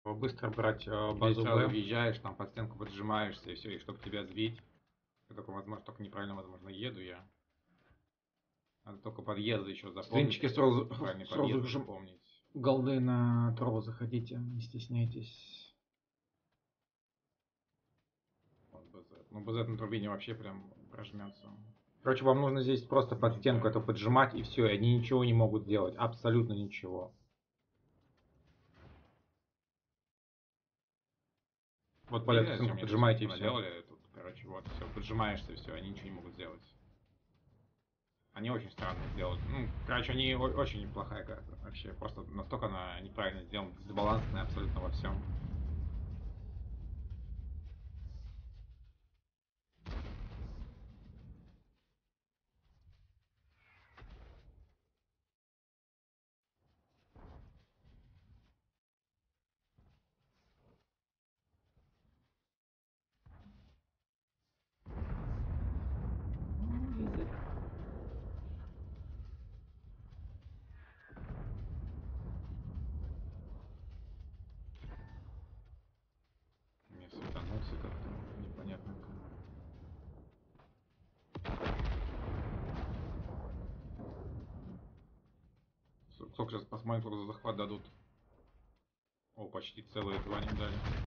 Как быстро взять базу Б | фата моргана
Привет, показываю Как быстро взять базу. новая карта фата-моргана.

Каждые 100 подписов - голда.